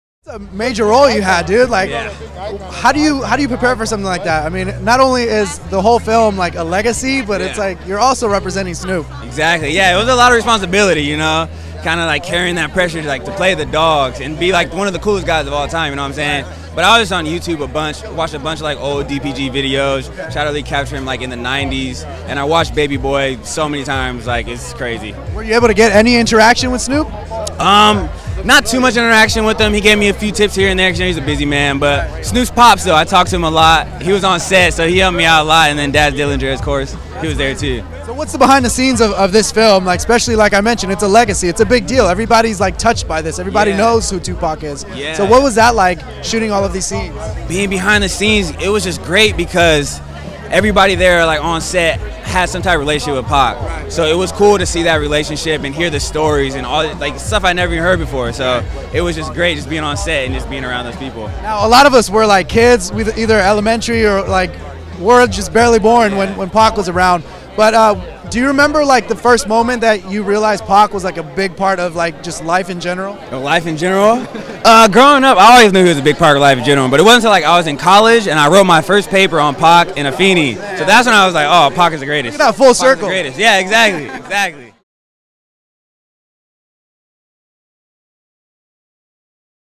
Jarrett Ellis on Becoming Snoop in Tupac Shakur’s ‘All Eyez On Me’ Biopic
Jarrett Ellis caught up with Rob Herrera at the Hollywood premiered of Tupac Shakur’s ‘All Eyez On Me’ and spoke about becoming Snoop for the film.